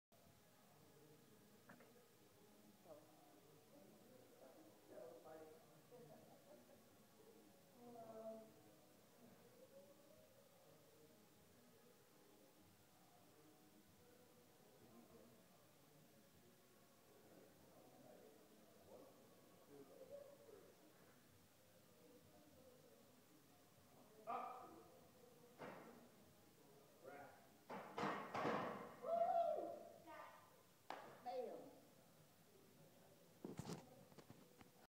235 raw bench @ Pro Gym
Training for Relentless March 23 and Donnie Thompson's SC meet April 6th..

Coaches Joe Somma & Josh Bryant